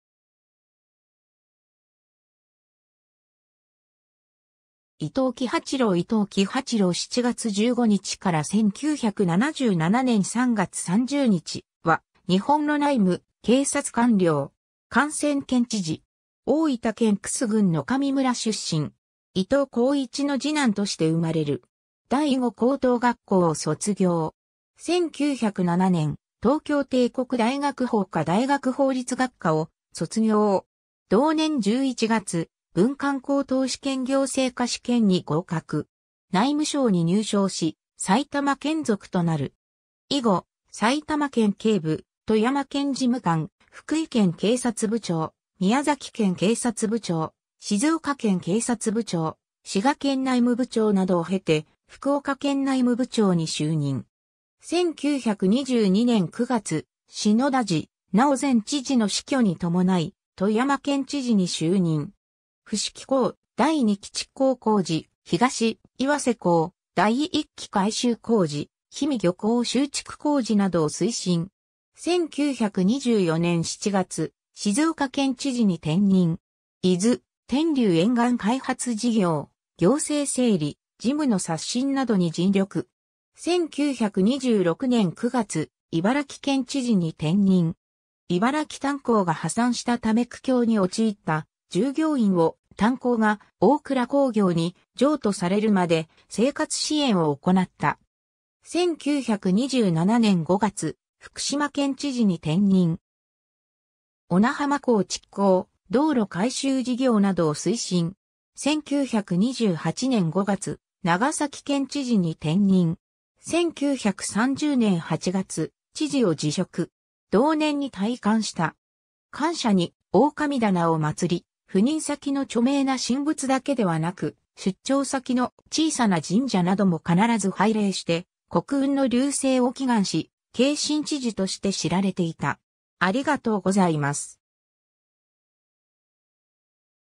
伊東喜八郎
伊東喜八郎 伊東 喜八郎（いとう きはちろう、1882年（明治15年）7月15日 - 1977年（昭和52年）3月30日）は、日本の内務・警察官僚。
官選県知事。
 大分県玖珠郡野上村（現在の九重町）出身。
伊東孝市の二男として生まれる。
第五高等学校を卒業。
1907年、東京帝国大学法科大学法律学科（独法）を卒業。
同年11月、文官高等試験行政科試験に合格。
内務省に入省し埼玉県属となる。
 以後、埼玉県警部、富山県事務官、福井県警察部長、宮崎県警察部長、静岡県警察部長、滋賀県内務部長などを経て、福岡県内務部長に就任。
 1922年9月、信太時尚前知事の死去に伴い富山県知事に就任。
伏木港第二期築港工事、東岩瀬港第一期改修工事、氷見漁港修築工事などを推進。
1924年7月、静岡県知事に転任。
伊豆・天竜沿岸開発事業、行政整理、事務の刷新などに尽力。
1926年9月、茨城県知事に転任。
茨城炭鉱が破産したため苦境に陥った従業員を、炭鉱が大倉鉱業に譲渡されるまで生活支援を行った。
1927年5月、福島県知事に転任。
小名浜港築港、道路改修事業などを推進。
1928年5月、長崎県知事に転任。
1930年8月、知事を辞職。
同年に退官した。
 官舎に大神棚をまつり、赴任先の著名な神仏だけではなく、出張先の小さな神社なども必ず拝礼して国運の隆盛を祈願し、「敬神知事」として知られていた。